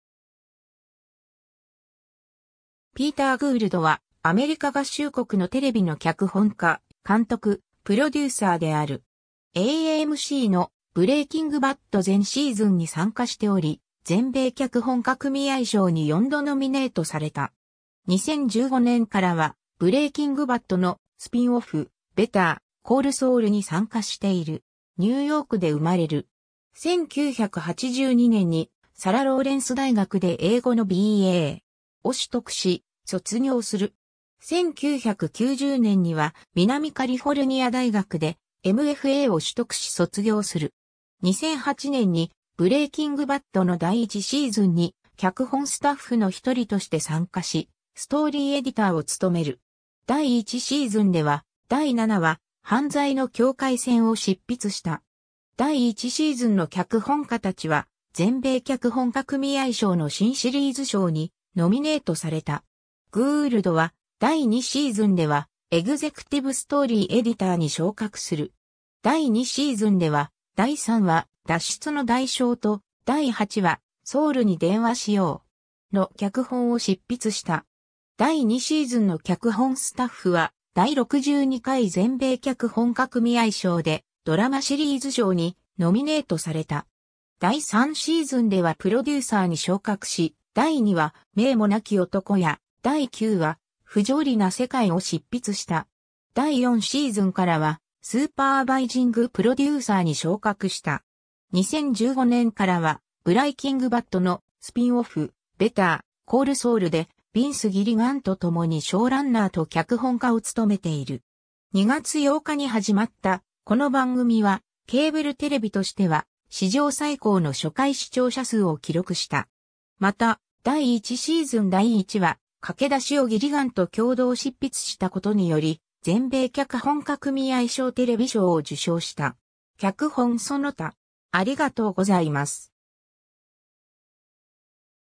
ピーター・グールド
ピーター・グールド（Peter Gould）は、アメリカ合衆国のテレビの脚本家、監督、プロデューサーである。
AMCの『ブレイキング・バッド』全シーズンに参加しており、全米脚本家組合賞に4度ノミネートされた。
2015年からは『ブレイキング・バッド』のスピンオフ『ベター・コール・ソウル』に参加している。
 ニューヨークで生まれる。
1982年にサラ・ローレンス大学で英語のB.A.を取得し、卒業する。
1990年には南カリフォルニア大学でM.F.A.を取得し卒業する。
 2008年に『ブレイキング・バッド』の第1シーズンに脚本スタッフの1人として参加し、ストーリー・エディターを務める。
第1シーズンでは第7話「犯罪の境界線」を執筆した。
第1シーズンの脚本家たちは全米脚本家組合賞の新シリーズ賞にノミネートされた。
 グールドは第2シーズンではエグゼクティブ・ストーリー・エディターに昇格する。
第2シーズンでは第3話「脱出の代償」と第8話「ソウルに電話しよう!」の脚本を執筆した。
第2シーズンの脚本スタッフは第62回全米脚本家組合賞でドラマシリーズ賞にノミネートされた。
第3シーズンではプロデューサーに昇格し、第2話「名もなき男」や第9話「不条理な世界」を執筆した。
第4シーズンからはスーパーバイジング・プロデューサーに昇格した。
 2015年からは『ブライキング・バッド』のスピンオフ『ベター・コール・ソウル』でヴィンス・ギリガンと共にショーランナーと脚本家を務めている。
2月8日に始まったこの番組は、ケーブルテレビとしては史上最高の初回視聴者数を記録した。
また第1シーズン第1話「駆け出し」をギリガンと共同執筆したことにより、全米脚本家組合賞テレビ賞（エピソディック・ドラマ部門）を受賞した。
 脚本 その他